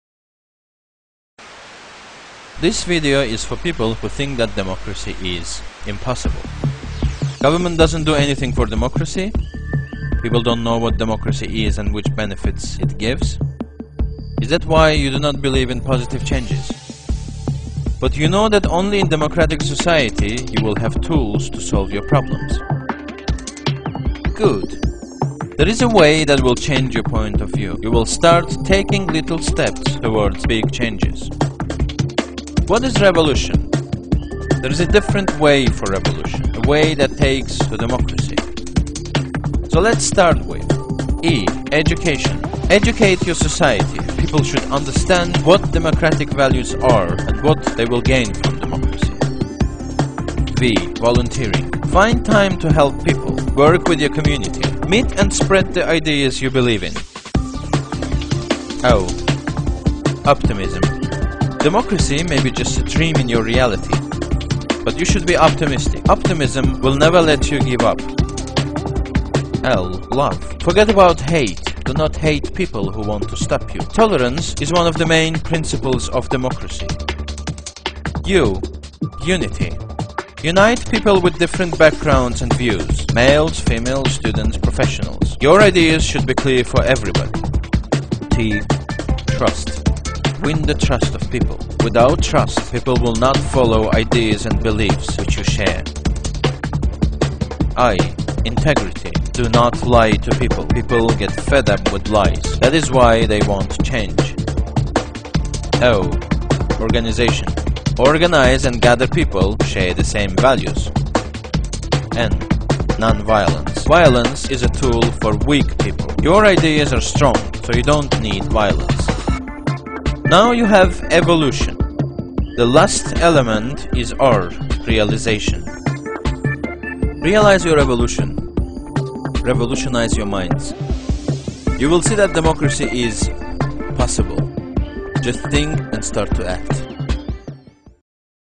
Democracy is... POSSIBLE
This video is for people who think that democracy is impossible.

Realize your evolution, revolutionize your minds.

You will see that democracy is POSSIBLE.

Just think and start to act!